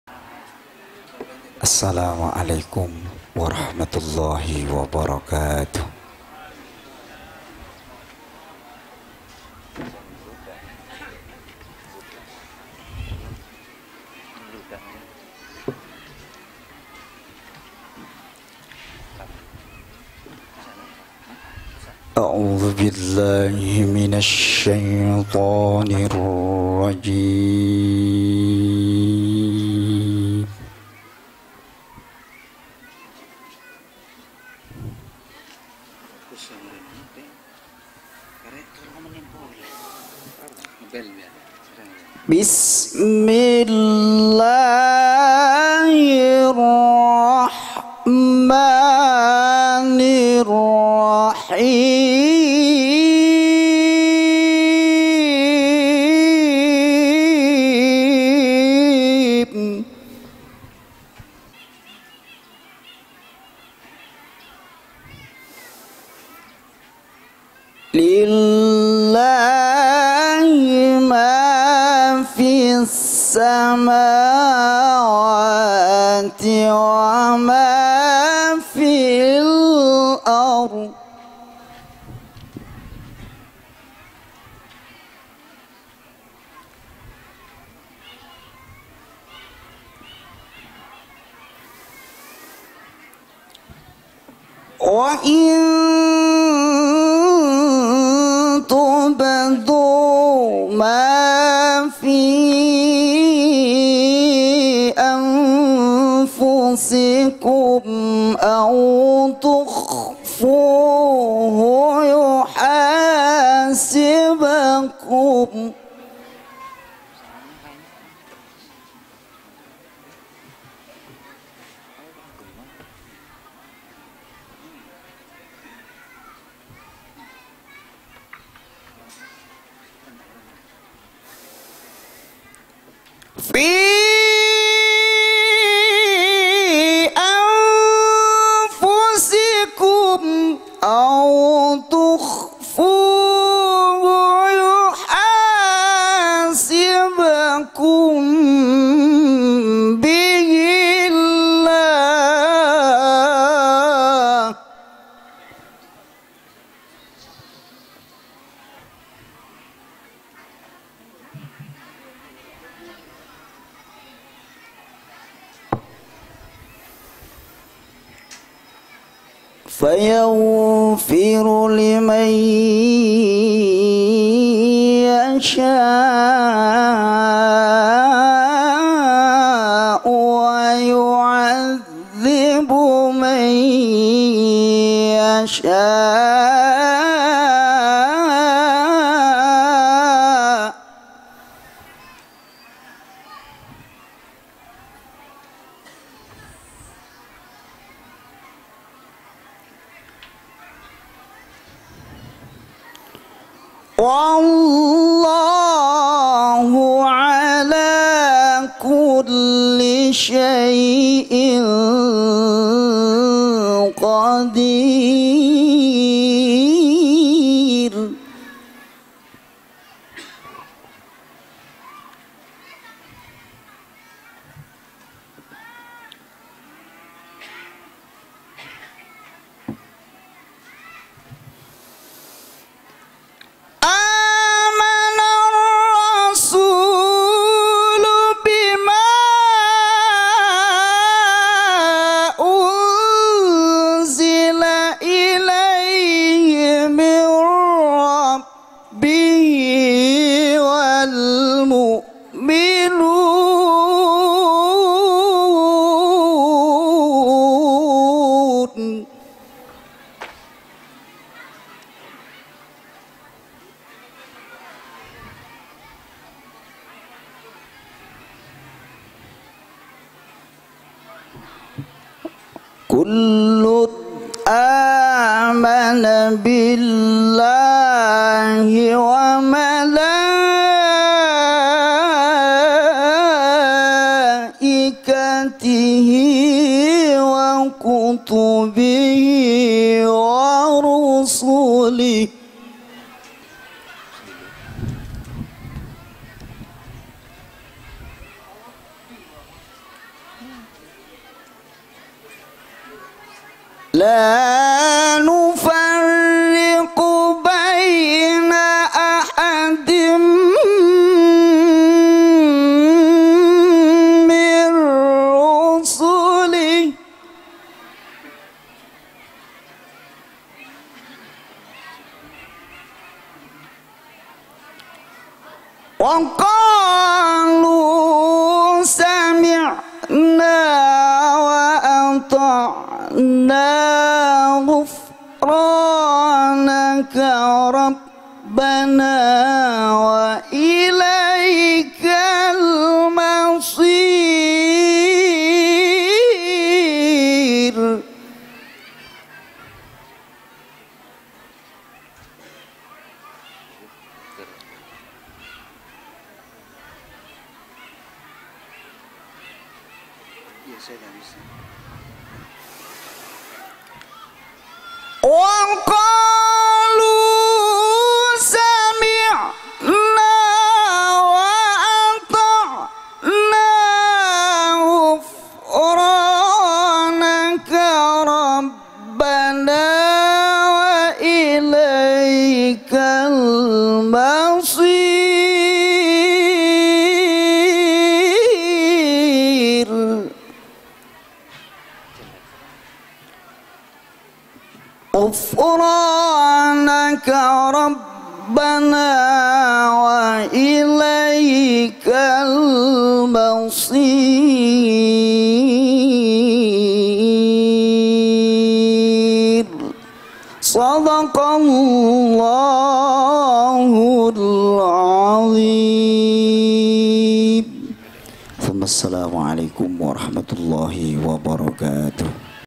THE BEST QORI' JAWATIMUR-USTADZ KHOIRUL UMAM
Pengajian Akbar HAUL MASYAIYIKH & HARLAH PP.DARUSSALAM KALIBARU - BANYUWANGI KE 67. AHAD 21 MEI 2017